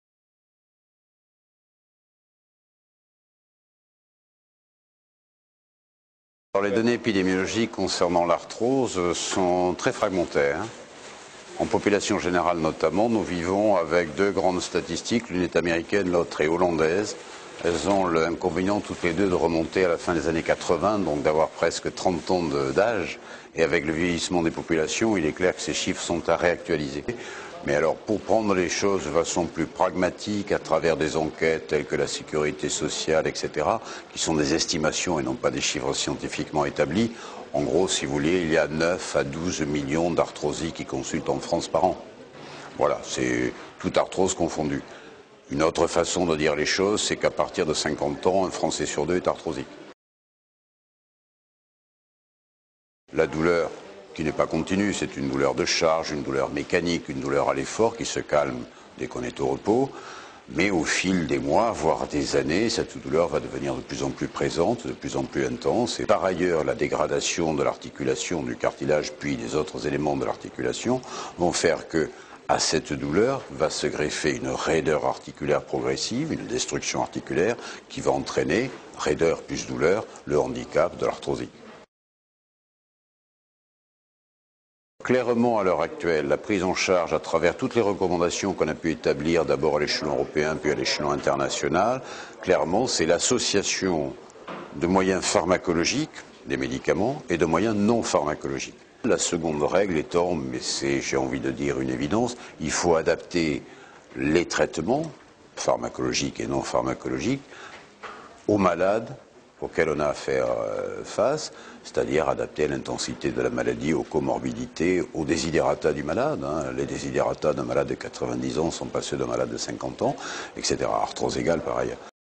L'arthrose touche une personne sur deux après 50 ans
Entre 9 et 12 millions de Français consultent chaque année pour une arthrose. Généralement, une seule articulation est touchée : la hanche, le genou et la colonne vertébrale sont les plus touchées. Le Pr. Bernard Mazières, chef du service de rhumatologie de l'hôpital Rangueuil à Toulouse nous en dit plus sur cette maladie articulaire.